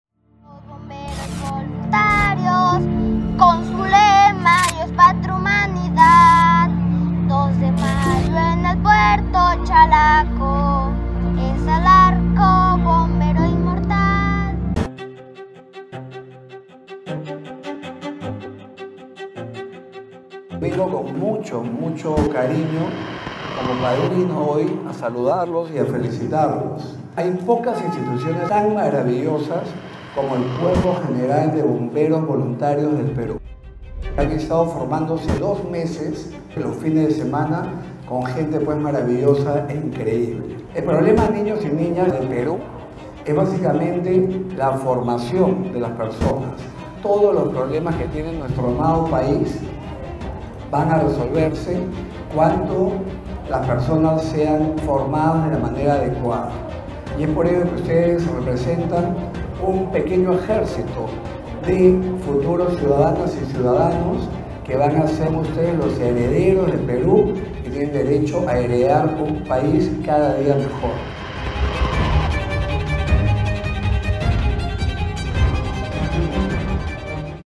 Clausura del curso de verano "Bomberitos 2024" de la Compañía de Bomberos N° 36
¡Celebramos juntos la clausura del curso de verano "Bomberitos 2024" de la Compañía de Bomberos N° 36! Un total de 56 "Bomberitos", entre niños y jóvenes, han adquirido conocimientos fundamentales sobre prevención de incendios y primeros auxilios. 🚒🔥 ¡Estamos orgullosos de su dedicación y compromiso con la seguridad!